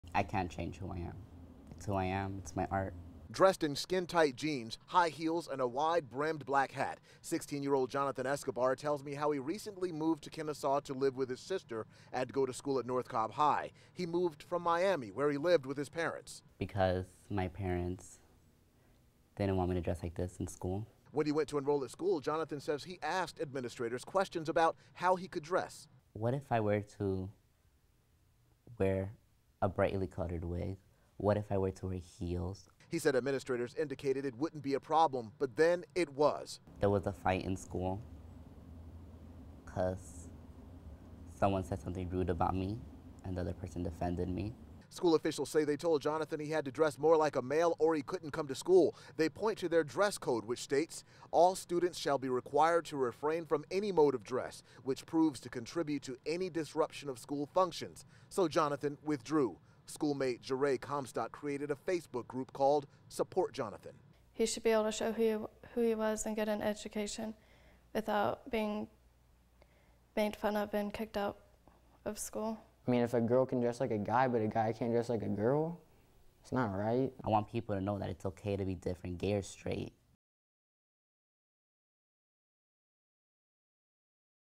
Cross Dressing Teen Jonathan Escobar (ORIGINAL the real story)
Kid withdrawls from school after school says he can not dress like a girl